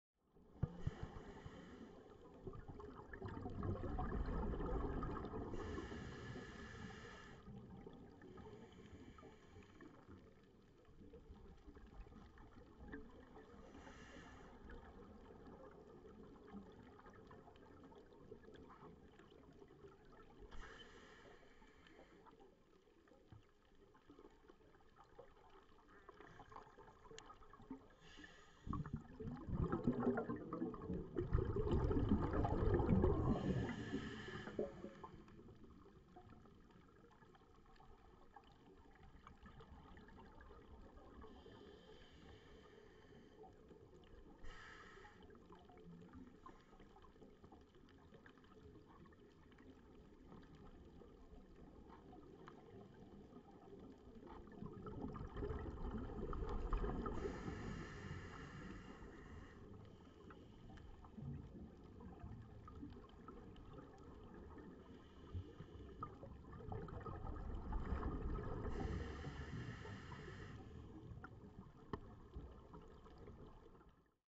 Massive Loggerhead Turtle
Captured encounter with massive loggerhead turtle